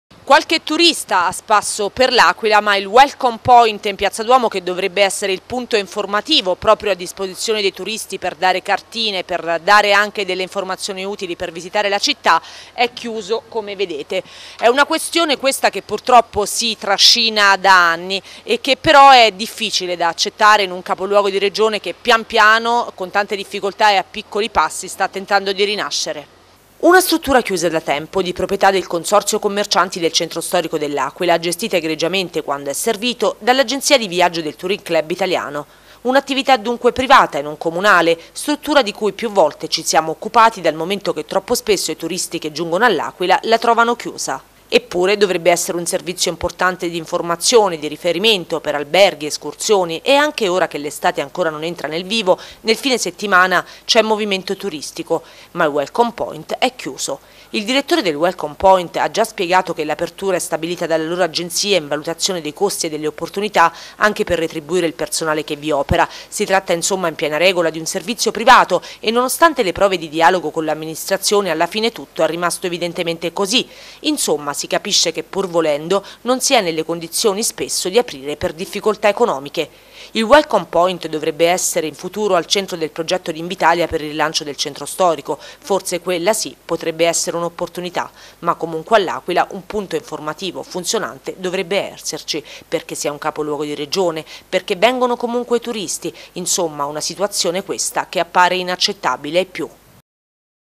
L’Aquila, chiuso il “Welcome point”
Welcom Point in Piazza Duomo chiuso. I turisti si lamentano ma la società che gestisce risponde: “E’ una struttura privata e non comunale, abbiamo difficoltà anche per i costi”. Di fatto il turista che raggiunge L'Aquila non ha punti di riferimento